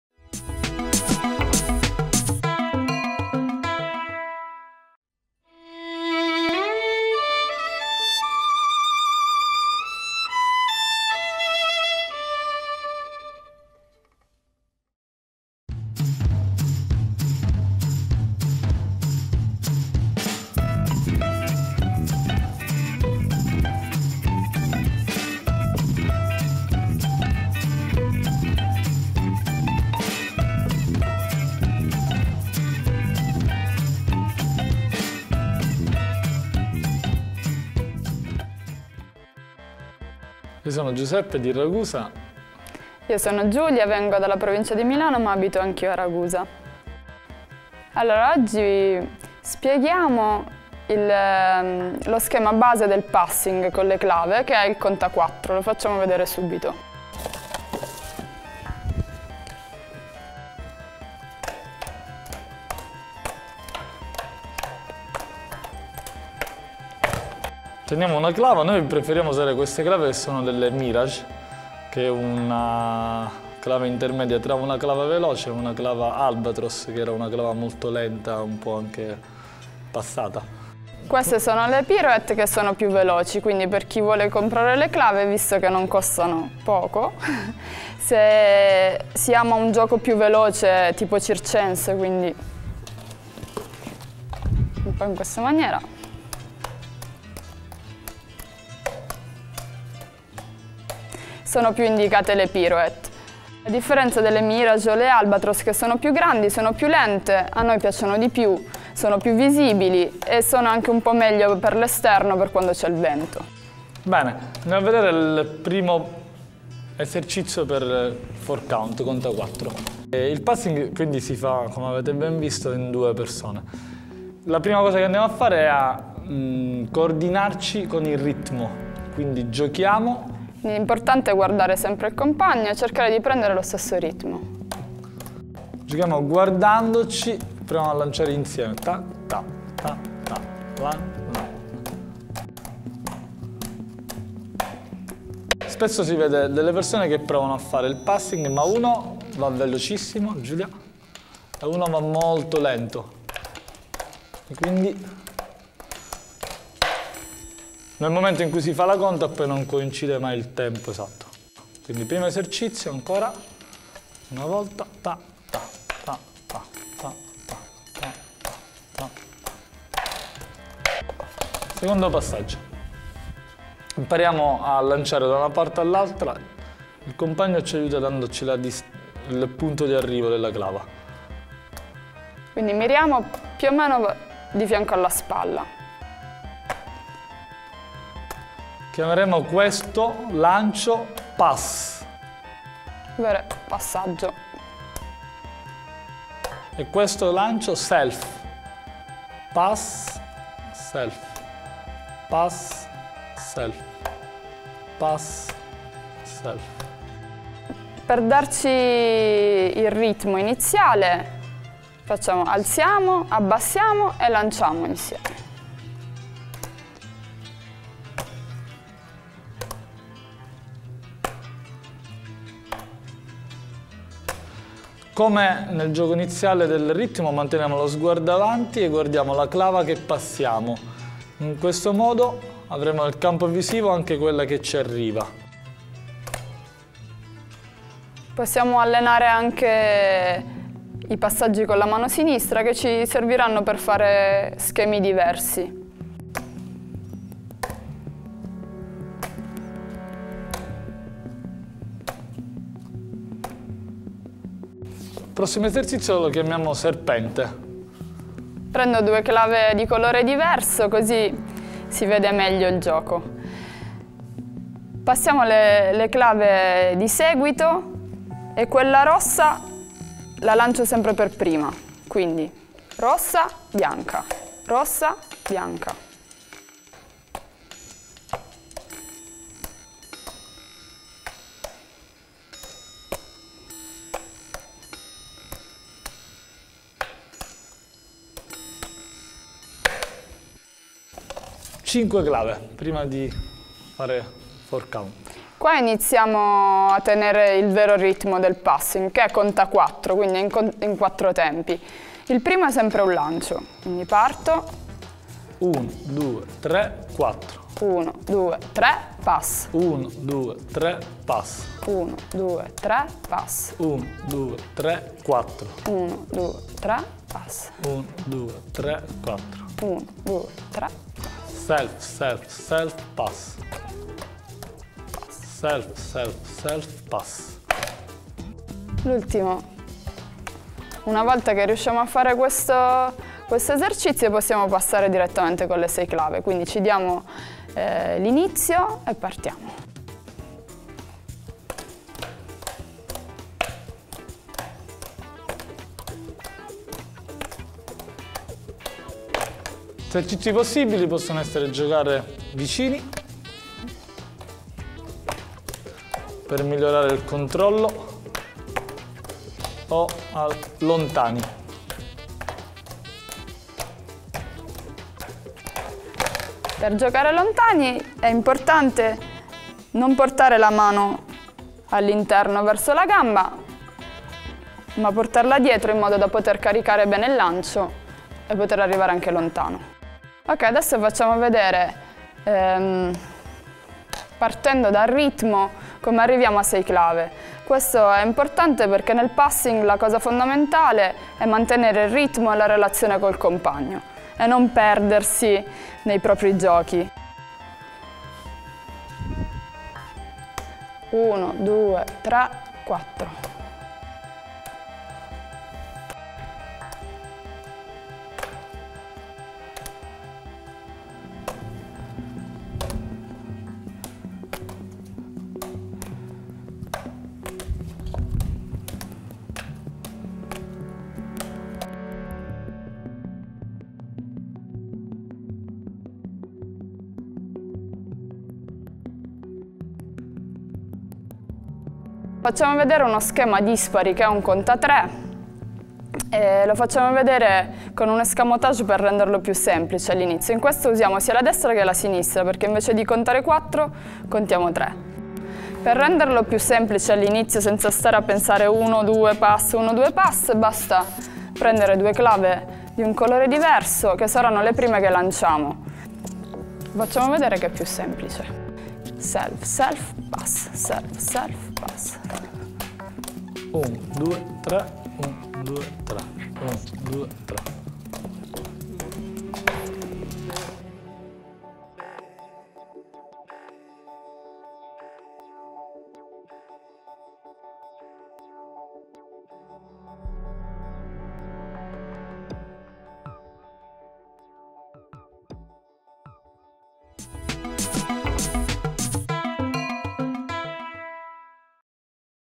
Lezioni di Gioco - Tutorial Clave con Peppe & Giù
Ad accompagnarci in una nuova lezione con le clave questa volta saranno Giuseppe Buggea e Giulia Treccosta, in arte Peppe e Giù. Artisti di strada e operatori di circo ludo-educativo, dal 2008
operano in diverse associazioni della provincia di Ragusa.

Informazioni sul tutorial
Passing con le clave:
movimenti base
lancio, direzione, presa.
2 clave, serpente
5 clave, self self self, pass alternato
6 clave four count (conta 4)
threecount conta 3.